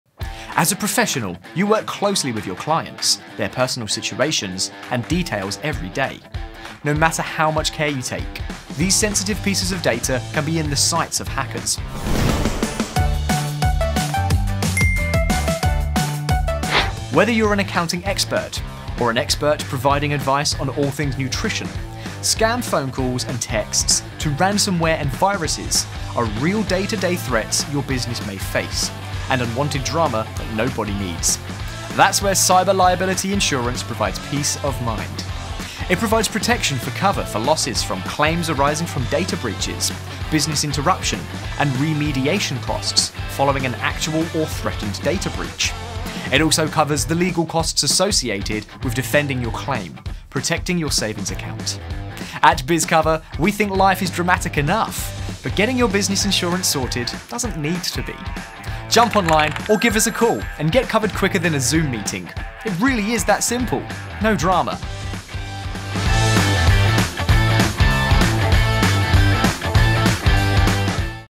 Insurance Under a Minute - Cyber Liability for Professionals
Whether you own a physical store or operate 100% online, Cyber Liability Insurance is something you should strongly consider as a Professional. 🖥️⚠️
Small businesses are an easy target for cyber criminals, so it's crucial that you have a Cyber Liability policy in place to protect your back pocket and reputation.